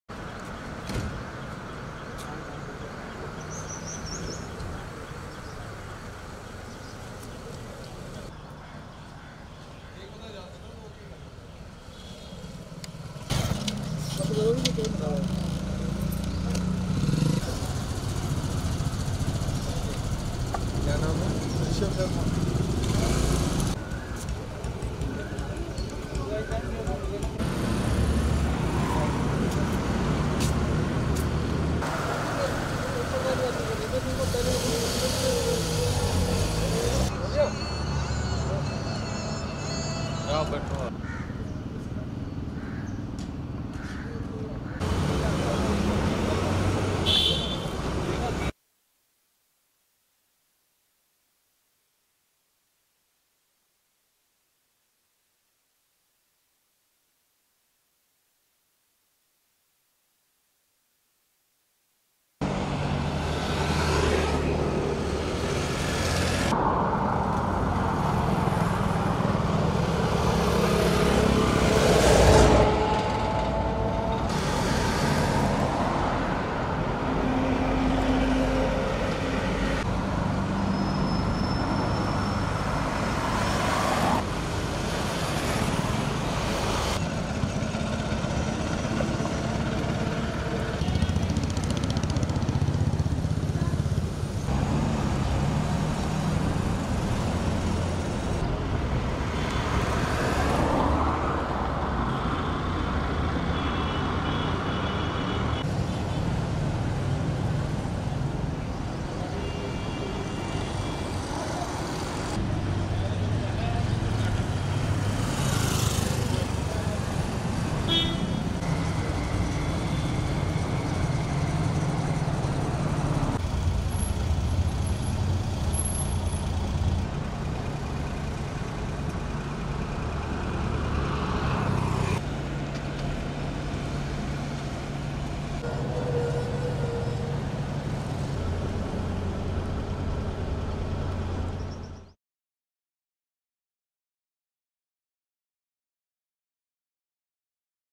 21-Day Lockdown: Jammu City Remains Shut On Day 14
| Excelsior News |

21-Day Lockdown: Jammu City Remains Shut On Day 14